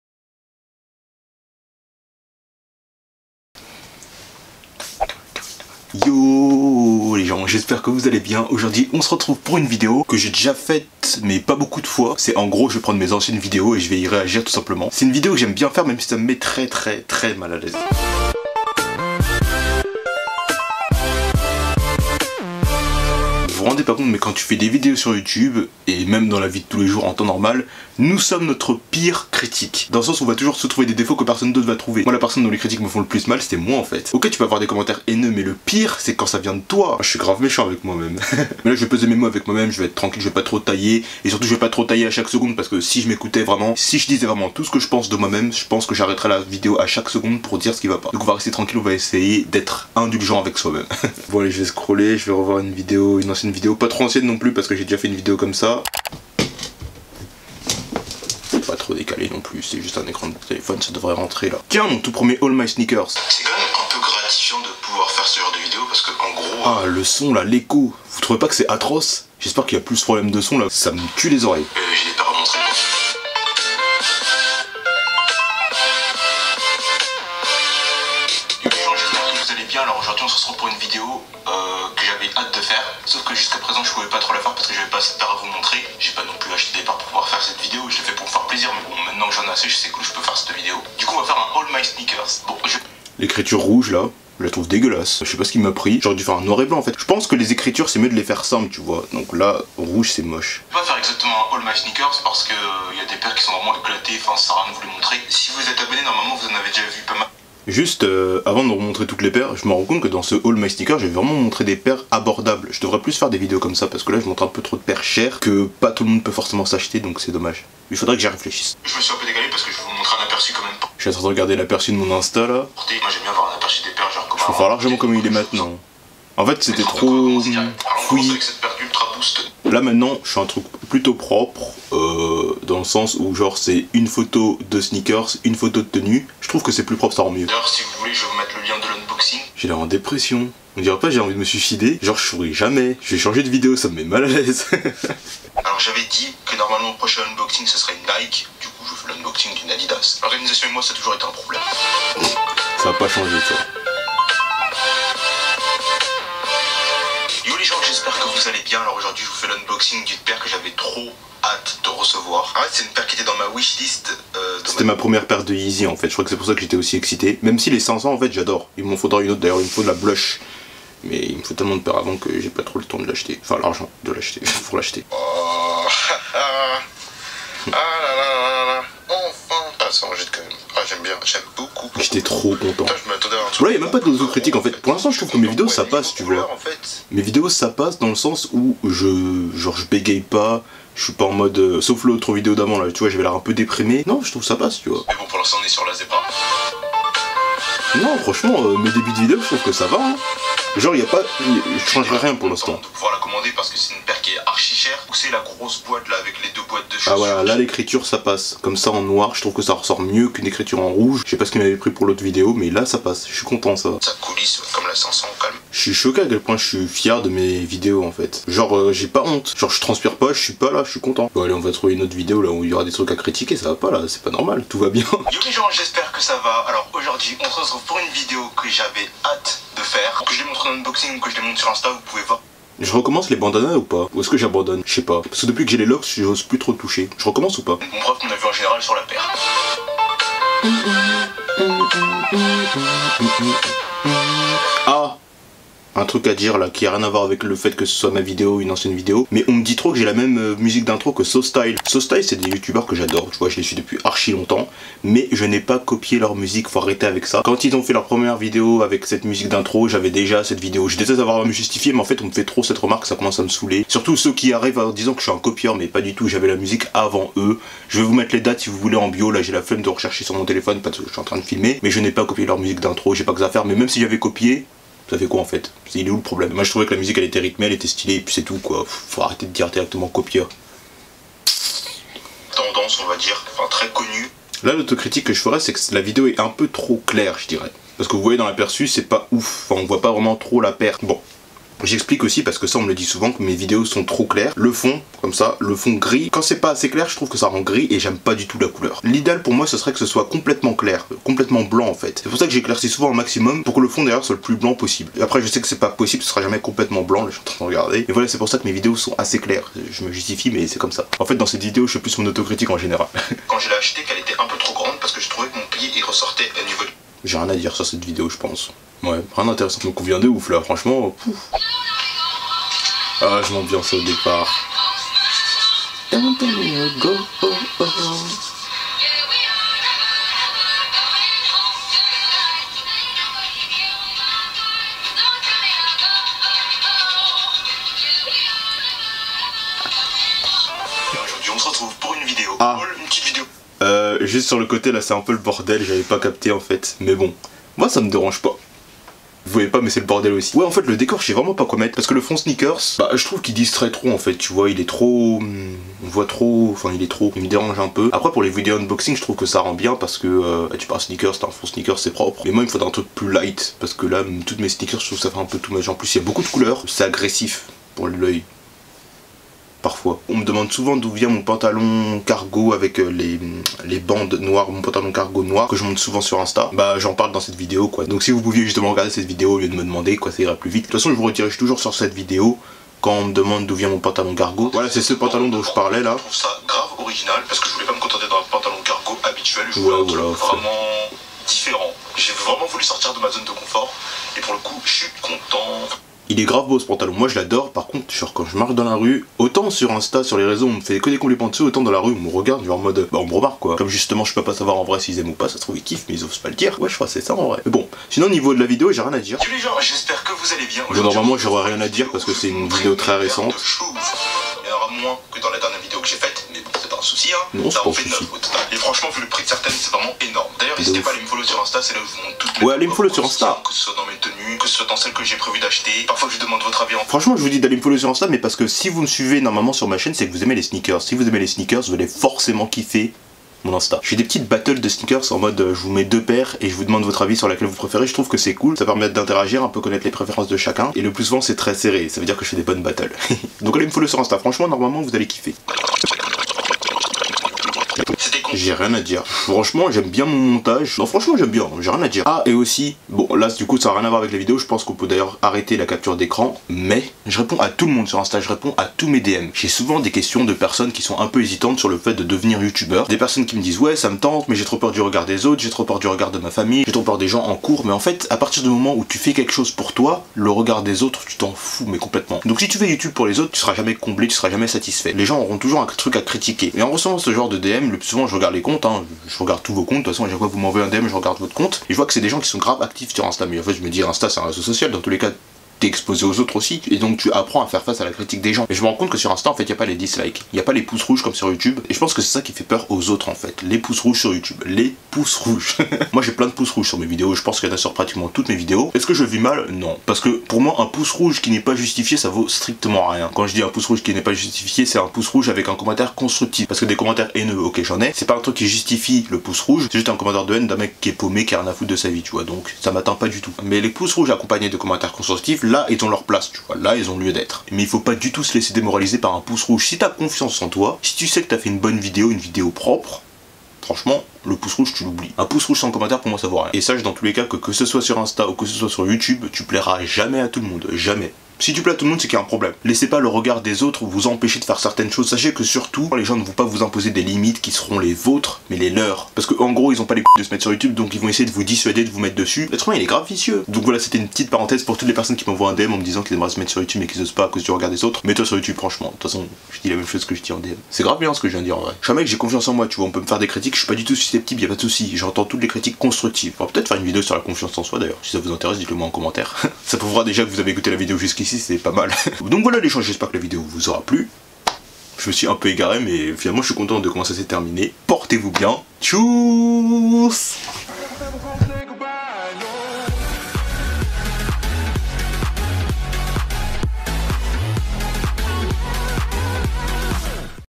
Je Réagis À Mes Anciennes Vidéos 🤔
J'espère avoir évolué un minimum... 😅 parce que sinan c'est chaud 😂